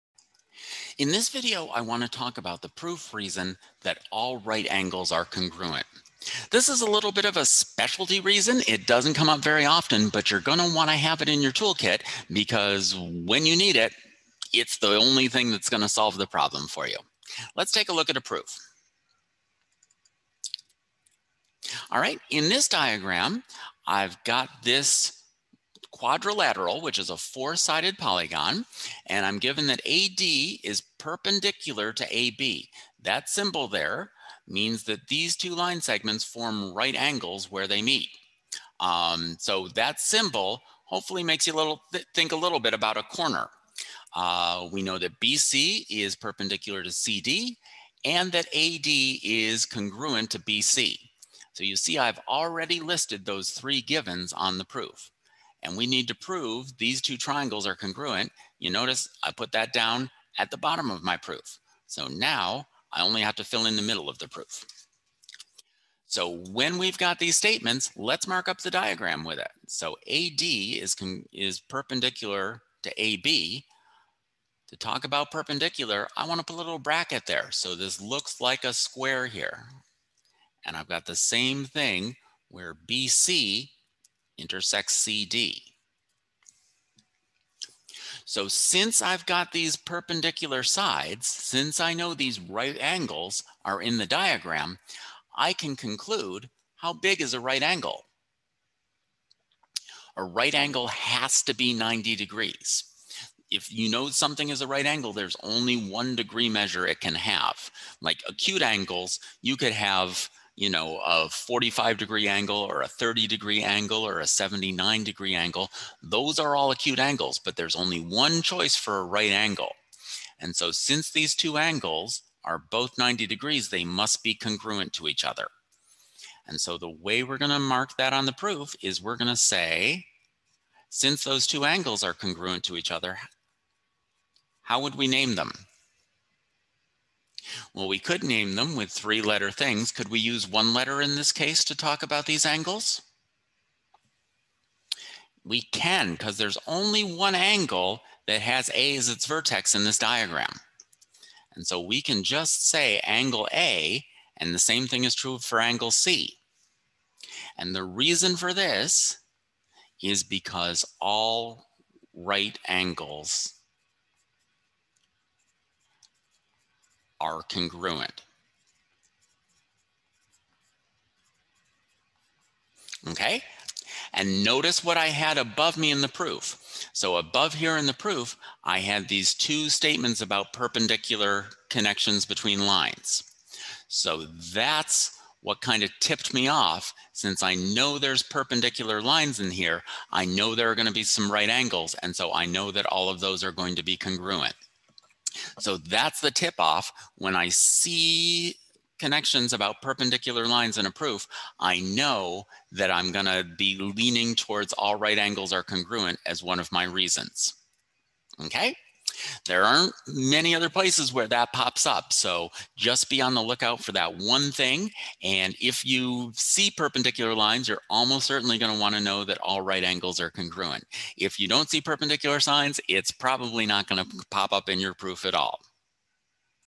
Proof Reasons - All Right Angles are Congruent
This video describes when we would want to use the reason "All Right Angles are Congruent" in a proof.